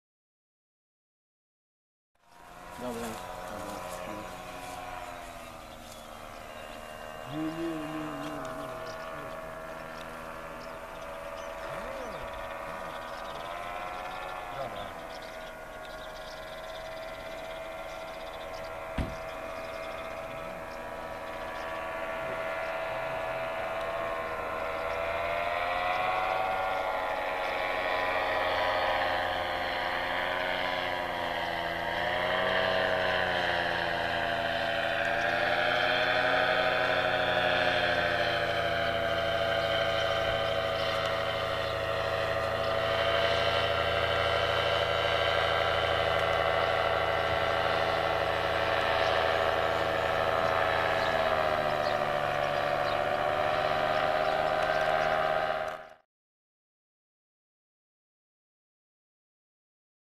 Motolotnią po Parku Narodowym
Przeloty motolotni  nad Słowińskim Parkiem Narodowym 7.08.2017r.